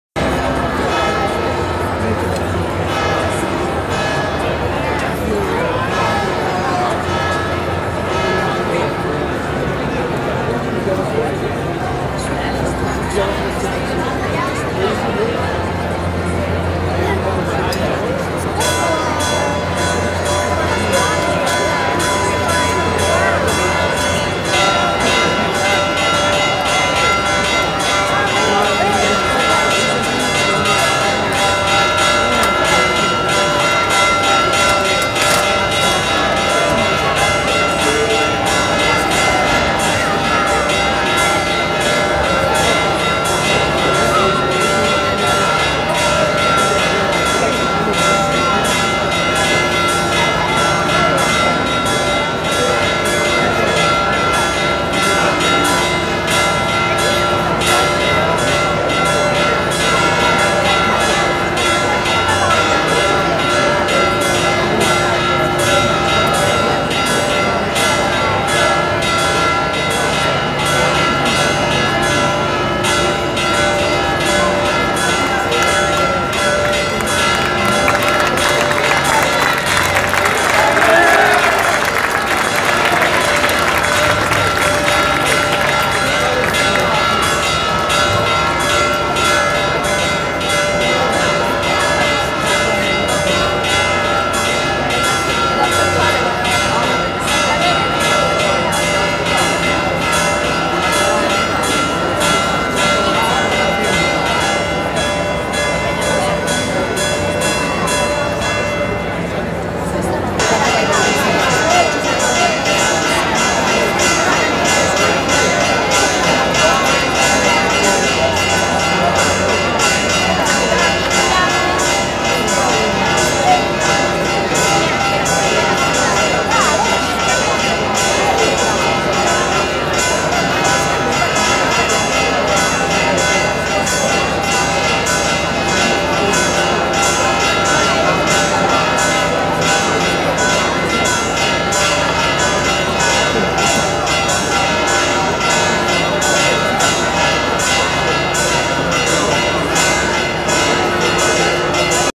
Acireale 26 Luglio 2023 Uscita dalla Basilica Cattedrale del Busto Reliquiario di Santa Venera 4K
Solenni Festeggiamenti in Onore di Santa Venera Vergine e Martire.
Gloriosa Patrona della Città di Acireale (CT).
Solenne Uscita dalla Monumentale Basilica Cattedrale della Processione dello Scrigno con le Reliquie e del Prezioso Busto Reliquiario di Santa Venera Vergine e Martire.
Piazza Duomo di Acireale (CT), Mercoledì 26 Luglio 2023 - Ore 21:00.
Buona Visione!!!